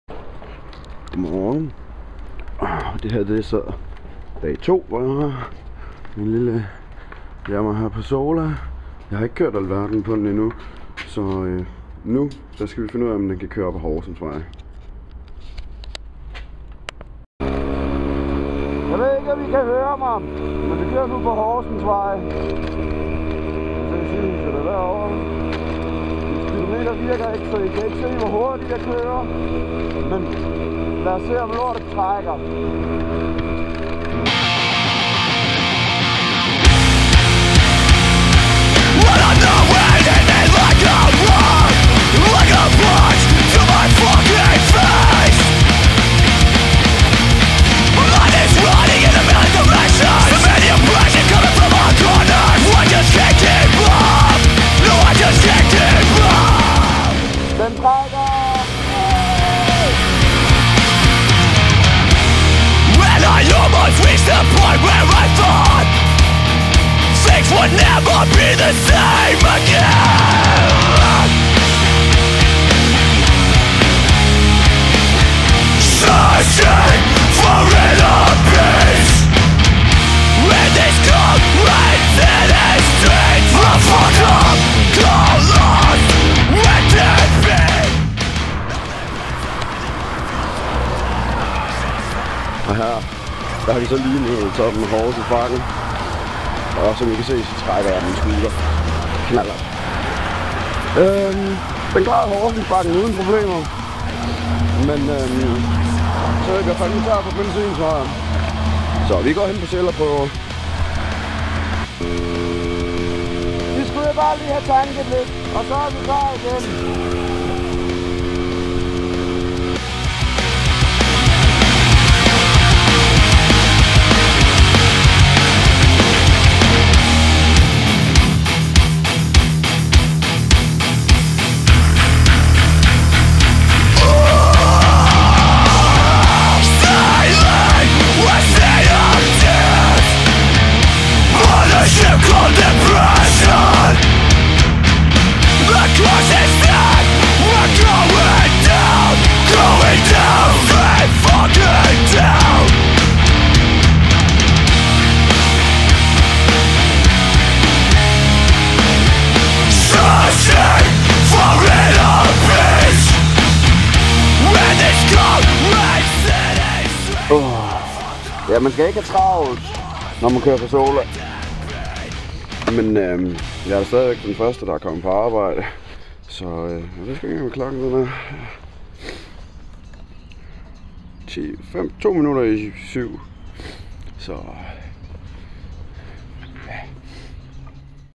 Passola Vs. Horsensvej pt. 1 [HD]
Ja, så her er første afsnit i føljetonen omkring min "nye" Yahama Passola. Denne gang er det LIGE efter jeg har fået den, og skal teste, om jeg overhovedet kan komme på arbejde på den. Næste video bliver, efter jeg har sat ny cylender på den, og så skal vi se hvor hurtigt den så trækker!